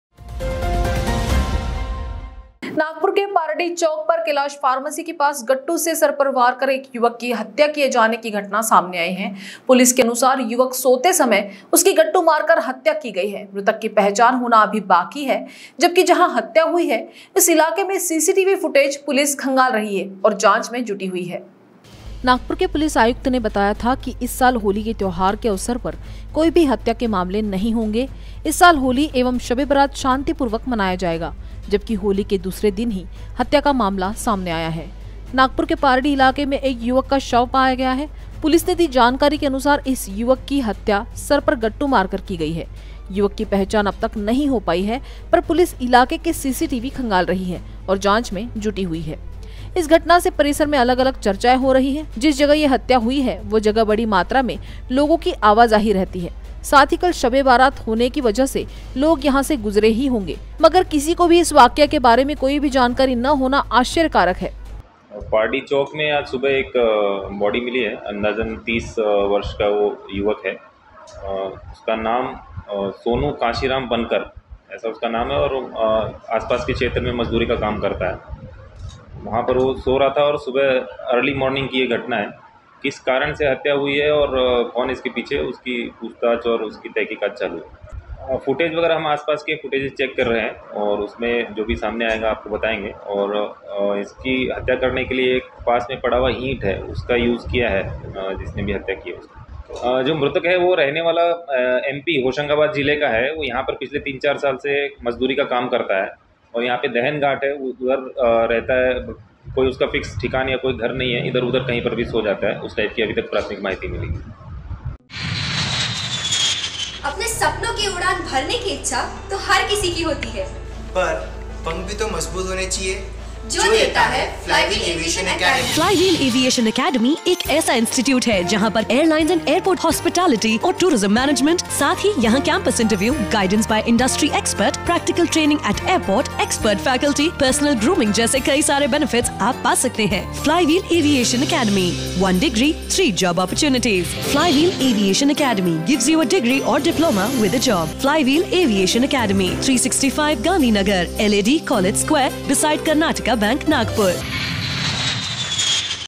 पारडी इलाके में मिली एक युवक की लाश
नागपुर के पारडी चौक पर कैलाश फार्मासि के पास  गट्टू से सर पर वार कर एक युवक की हत्या कीये जाने की घटना सामने आयी है. पोलिस के अनुसार युवक सोते समय उसकी गट्टू मारकर हत्या की गई. मृक्तक की पहचान होना अभी बाकी है जबकि, जहा हत्या हुई उस इलाके के सीसीटीवी फुटेज पुलिस खंगाल रही है और जांच में जुटी हुई है.
नागपूर के पुलिस आयुक्त ने बताया था की इस साल होली के त्यौहार के अवसर पर कोई भी हत्या के मामले नहीं होंगे इस साल होली एवं शब् इ बारात शांतिपूर्वक मनाया जाएगा जबकि होली के दूसरे ही दिन हत्या का मामला सामने आया है . नागपुर के पारडी इलाके में एक युवक का शव पाया गया. पुलिस ने दी जानकारी अनुसार इस युवक की हत्या सर पर गट्टू मारकर की गई है. युवक की पहचान अब तक नहीं हो पाई हैं पर पुलिस इलाके के सीसीटीवी खंगाल रही है और जांच में जुटी है. इस घटना से परिसर मे अलग अलग चर्चाएं हो रही है जिस जगह यह हत्या हुई वो जगह बड़ी मात्रा में लोगो की आवाजाही रहती है साथ ही कल शब ए बारात होने की वजह से लोग यहां से गुजरे ही होंगए मगर किसी के भी इस वाकये के बारे में कोई जानकारी न होना आश्चर्यकारक है।